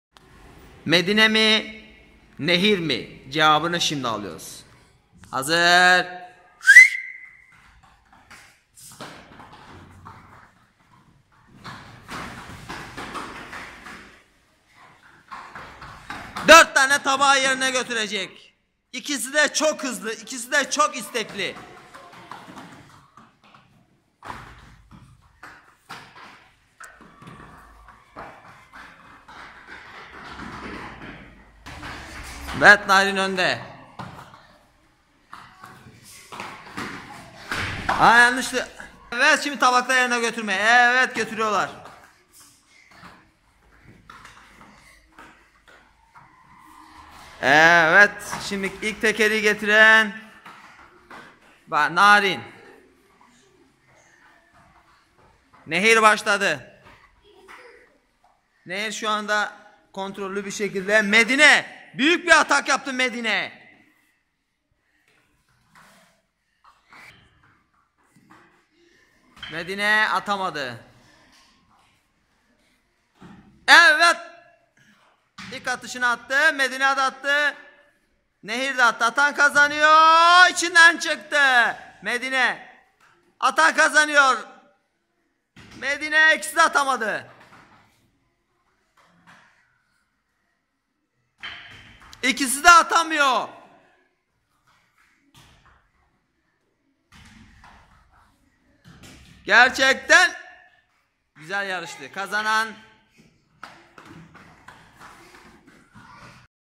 SURVİVOR ÇOCUKLAR YARIŞIYOR
KİCKBOKS KURSUMUZ parkuru yarışlarla devAm ediyor

KICKBOKS ANTRENMAN SONRASI YARIŞLARIMIZ DEVAM EDİYOR
-survivor
-yarışlar
-büyükkapışma